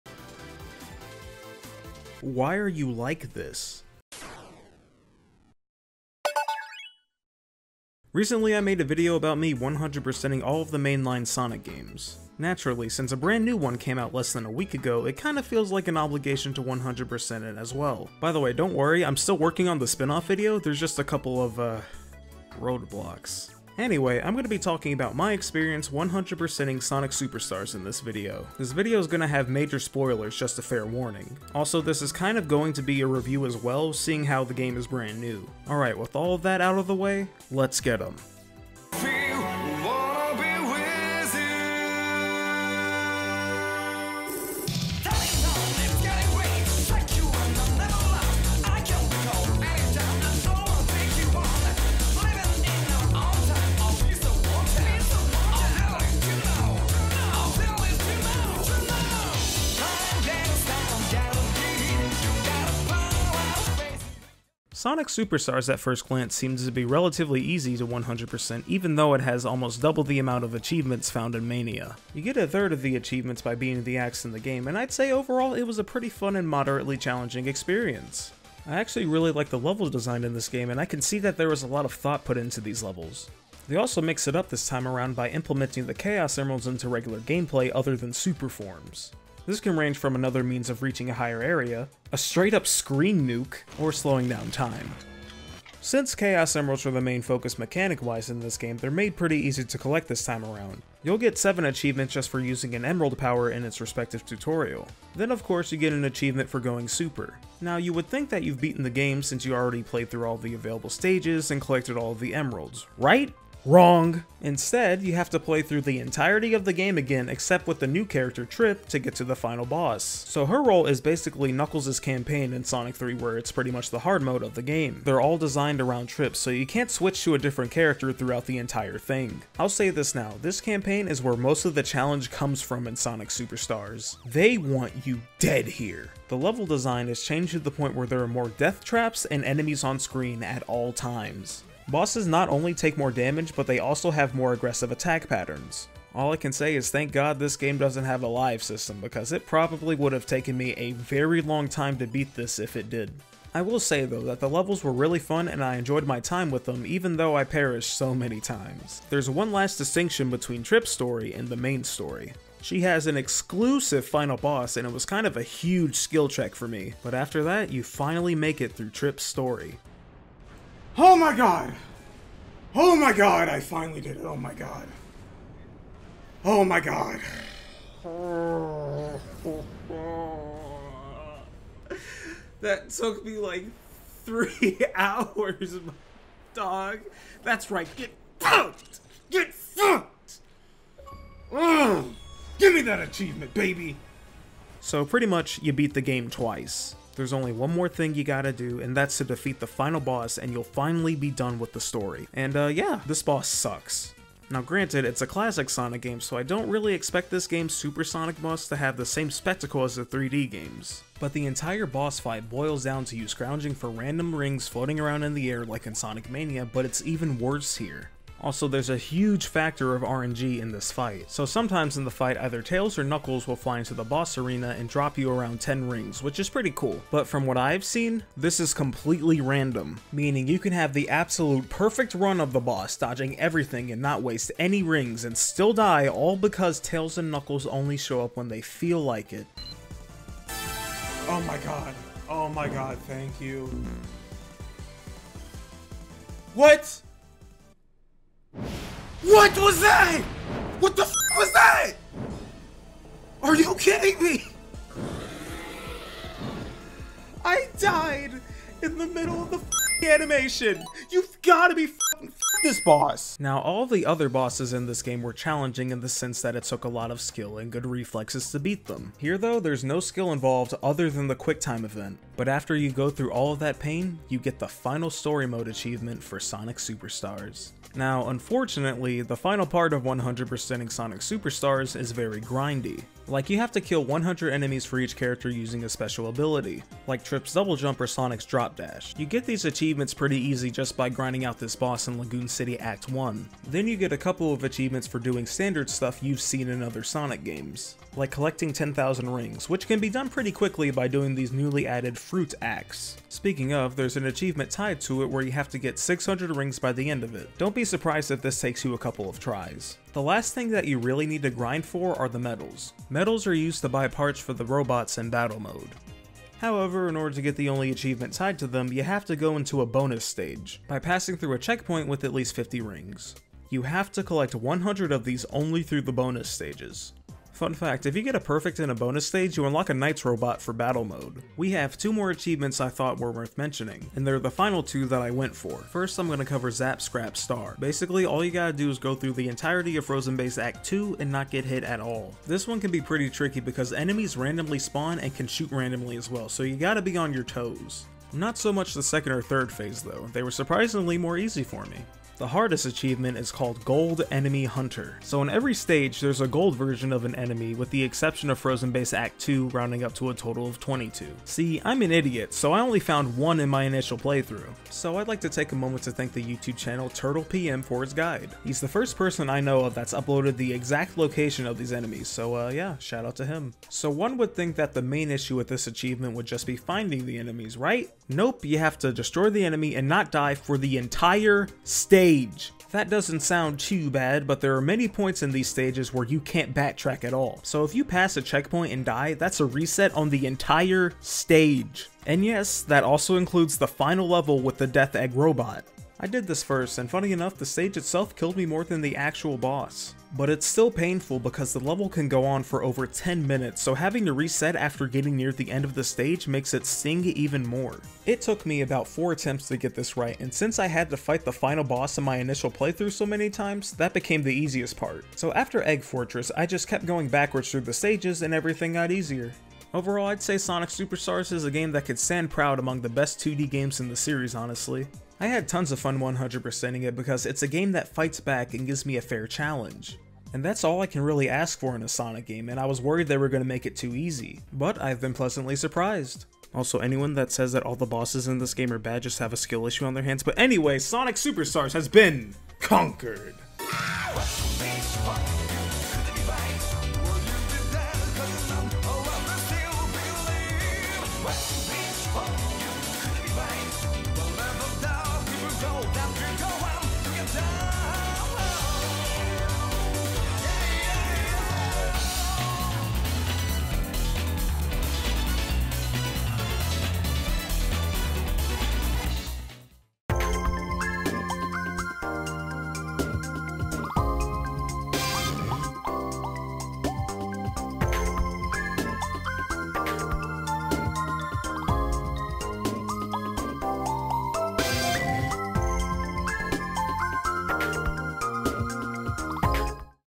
I 100 percented Sonic Superstars. Should You?
Long time no see it's Luxe Crux back at it with another video! In this video I talk about my experience 100 percenting the new Sonic game, Sonic Superstars!
Made the renders for the video thumbnail!
for making my character stills!

I'm Luxe Crux! You'll mostly find reviews, video essays, gameplay, challenges, or news. Thanks for stopping by!